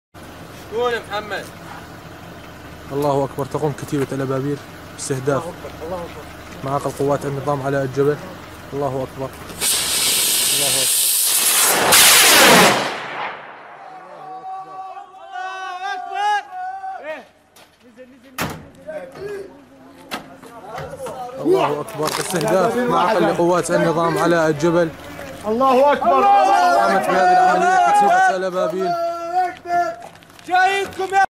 شام دير الزور استهداف مقرات جيش النظام بصواريخ محلية الصنع على يد الجيش الحر 4 2 2014 جـ2
مراسل شام في دير الزور ثائر الديري رصد استهداف الجيش الحر لمعاقل قوات النظام بصواريخ محلية الصنع.